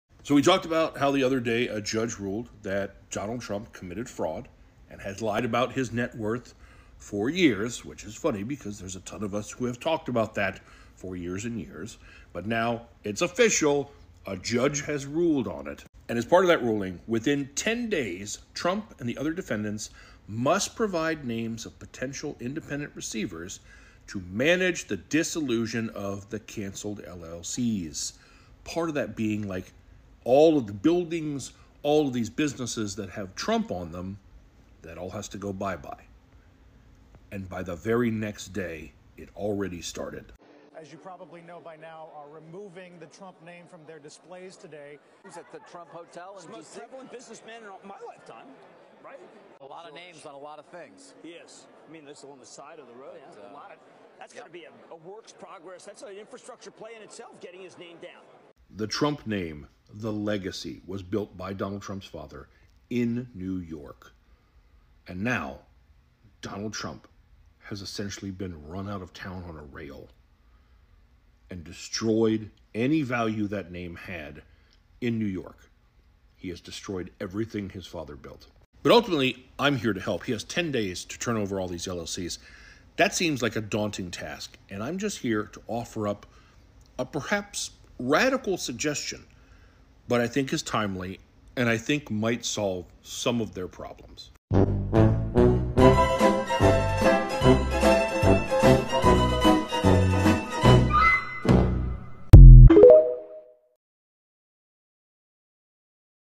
😲🗽:Trump👱🏻🕴🏻 Committed FRAUD 🤥 & Lied about his Wealth🏗️🌆🌁...The 👩‍⚖️Orders...
🤯Trump Caught & Busted...NYC 👩‍⚖️ Orders the shutting down of all Trump businesses.The Trump Signs are being removed from His buildings.Trump's father Fredrick Trump👴🏻is Completely DISGRACED,the one who founded the Trump Real Estate Empire.